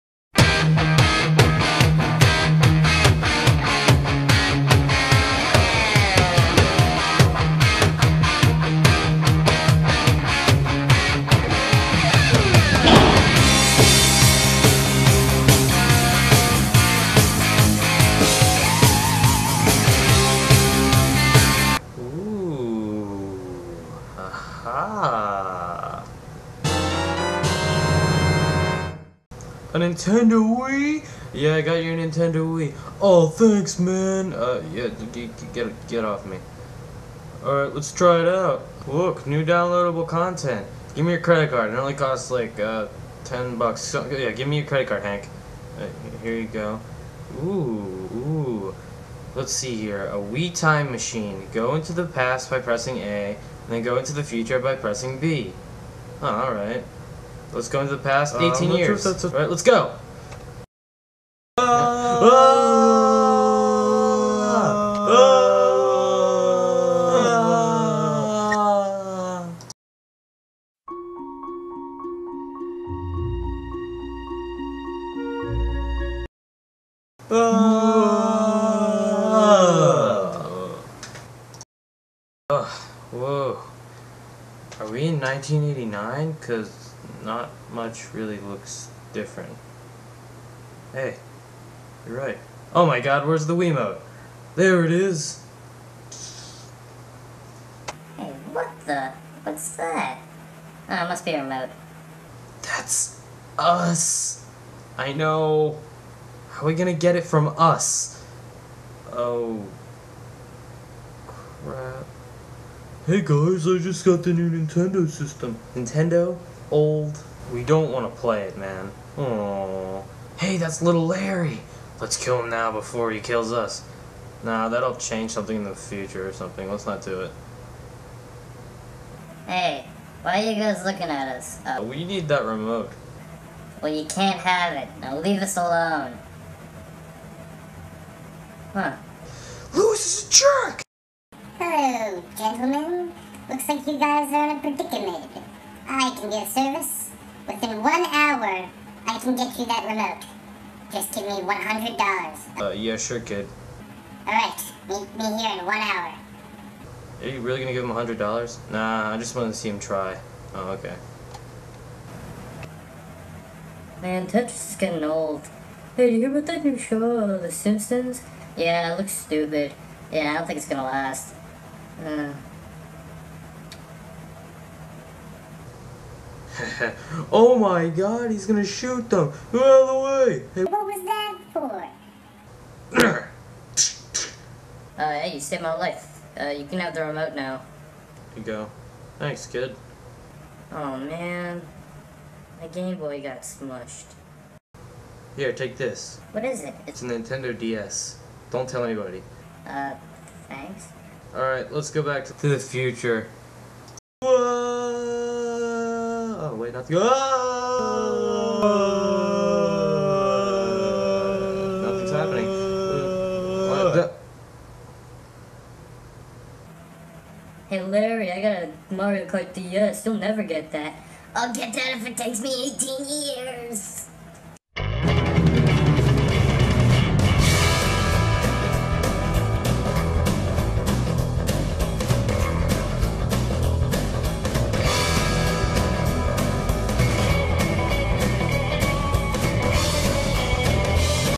Hank and Louis Episode 21: "Time Travel and the Wii"
Louis gets a time traveling wii.

im still deciding whether or not i should kept the newly designed hank and louis, or return to the original, SO PLEASE COMMENT saying yes or no!